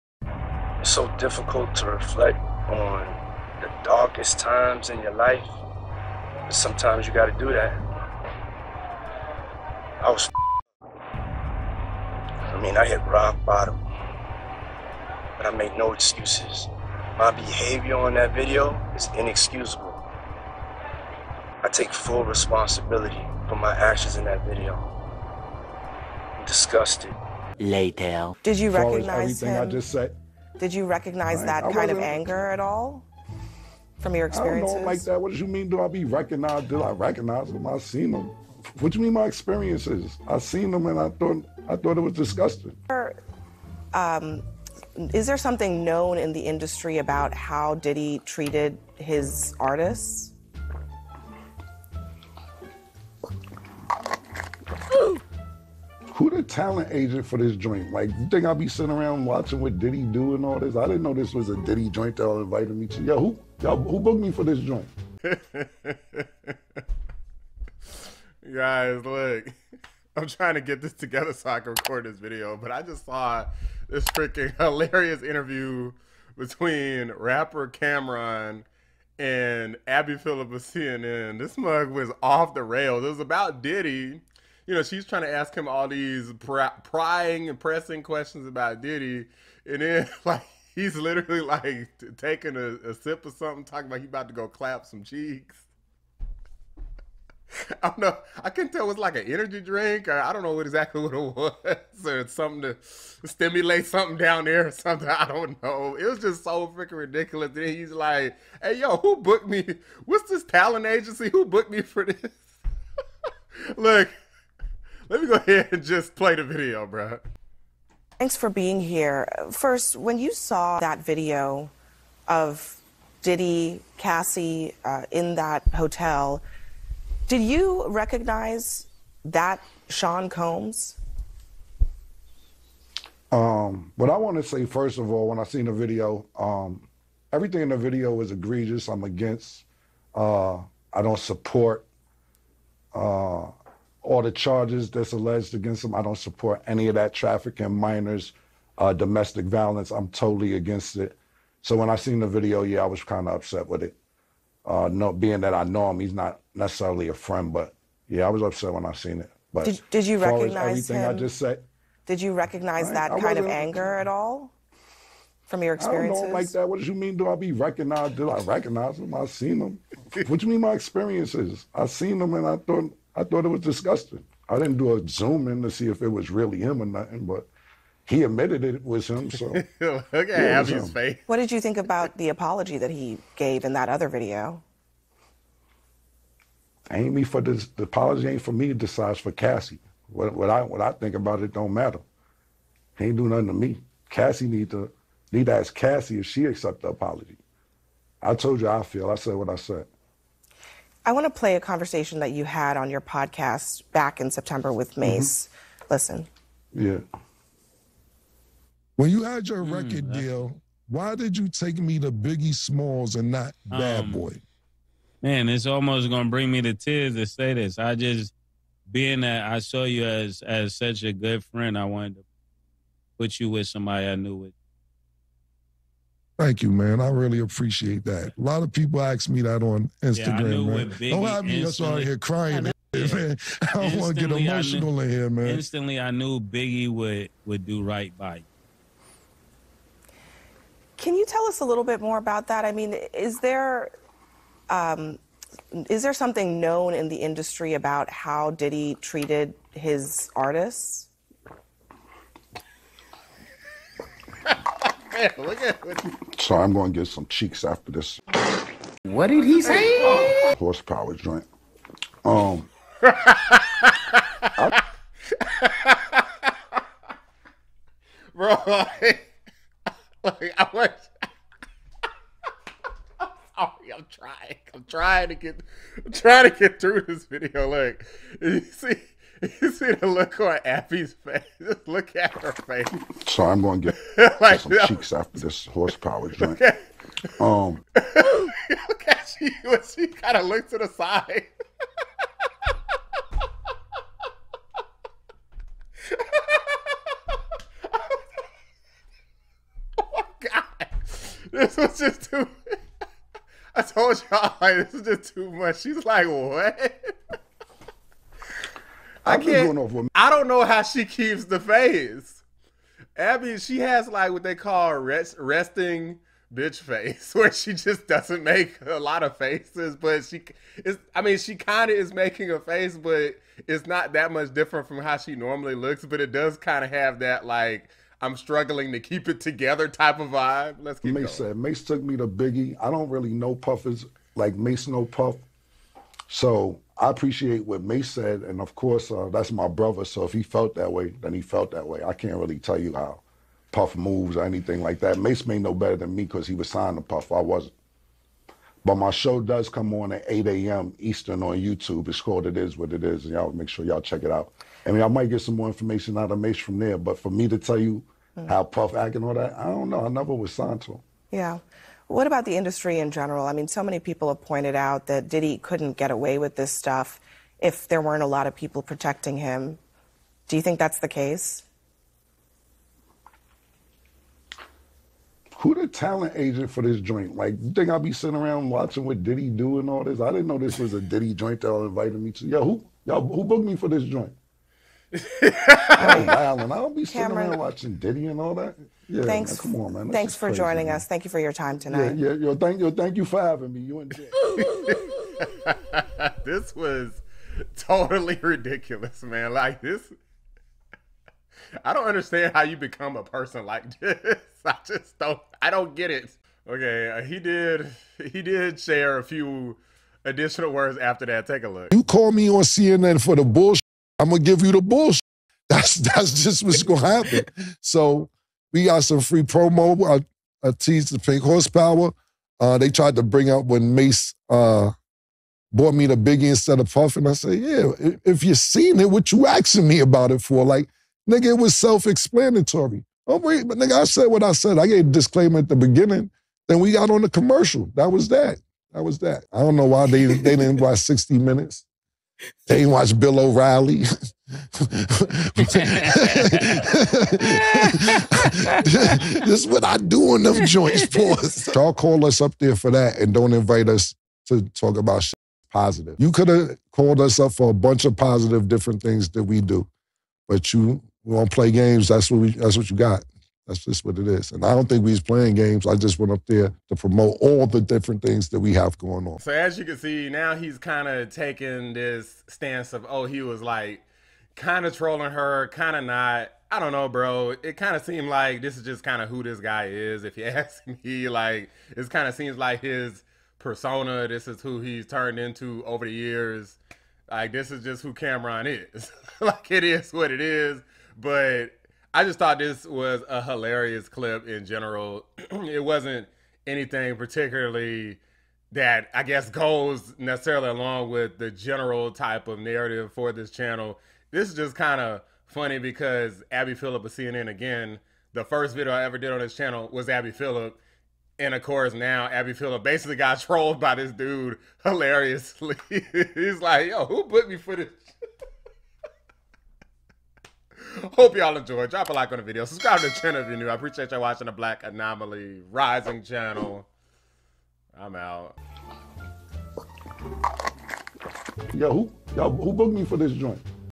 CNN Anchor Abby Phillip HUMILIATED By Rapper Cam'Ron In DISASTEROUS Diddy Interview!
CNN news anchor Abby Phillip was joined by rapper Cam'ron where she was HUMILIATED in what can only be described as a DISASTEROUS interview for asking him questions about his relationship with the controversy embroiled rapper, Diddy!